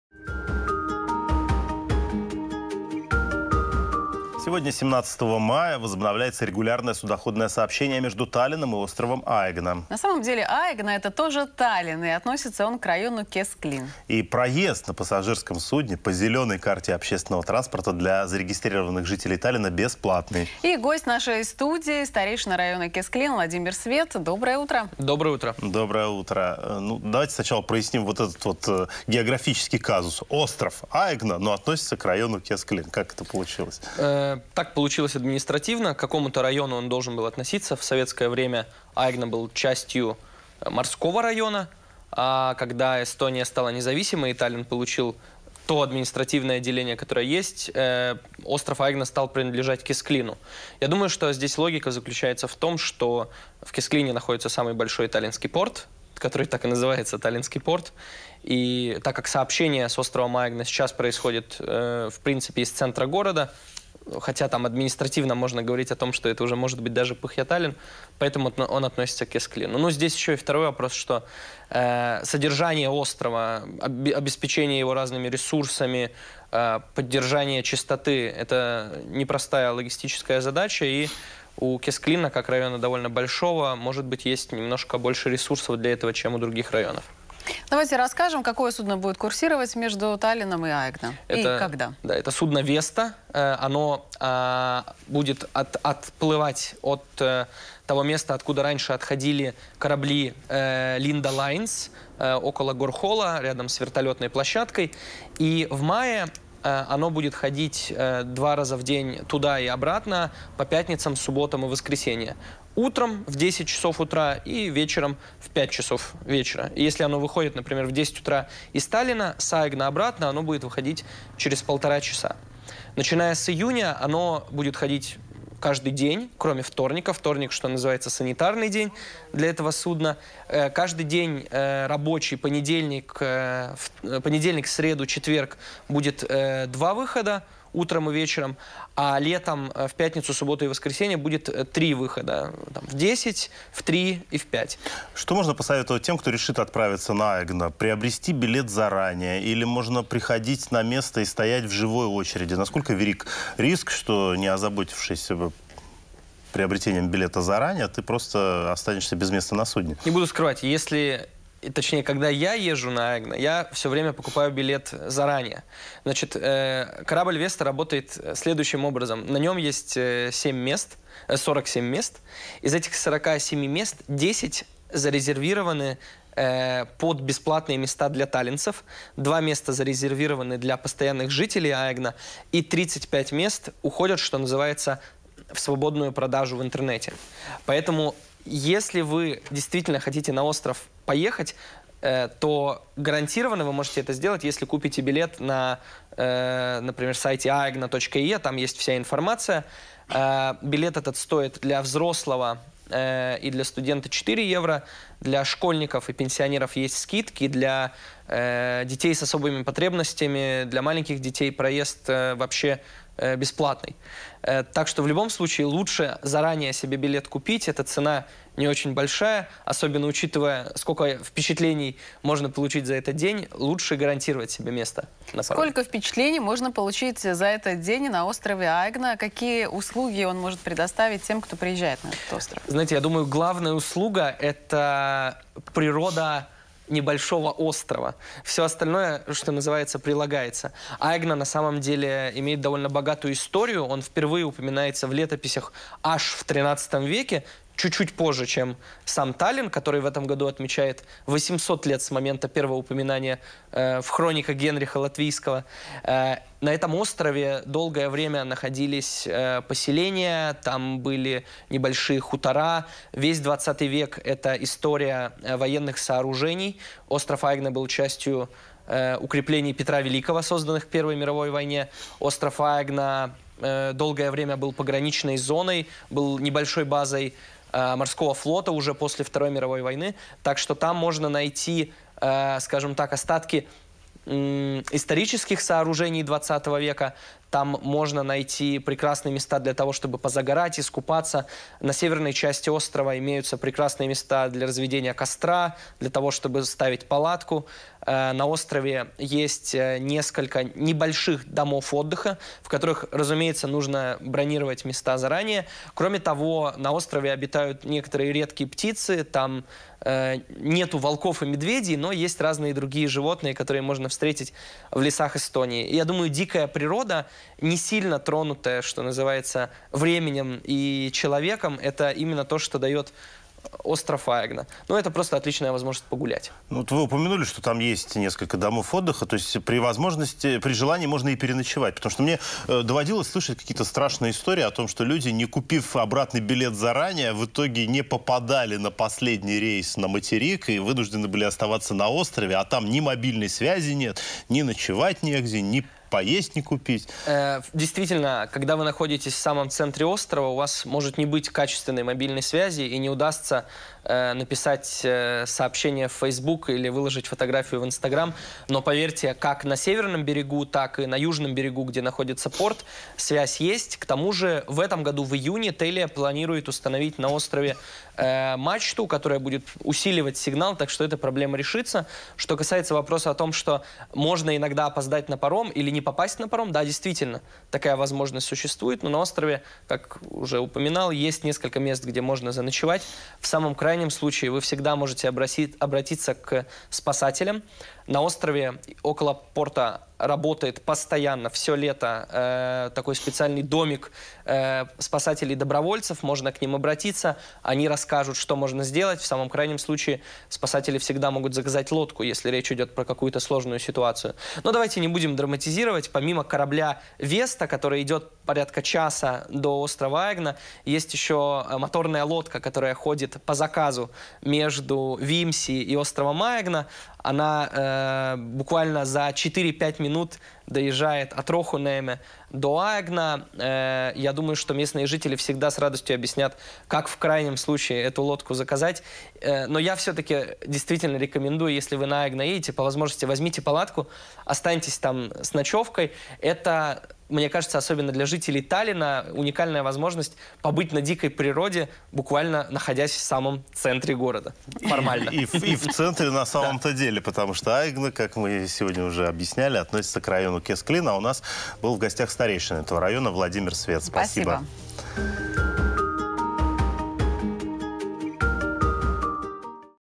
Аэгна — это центр Таллинна! Как доехать и что делать на острове?
Сегодня, 17 мая, возобновляется регулярное судоходное сообщение между Таллинном и островом Аэгна. На самом деле, Аэгна — это тоже Таллинн, и относится он к району Кесклинн. И проезд на пассажирском судне по зеленой карте общественного транспорта для жителей Таллинна бесплатный. Гость нашей стадии — старейшина района Кесклинн Владимир Свет.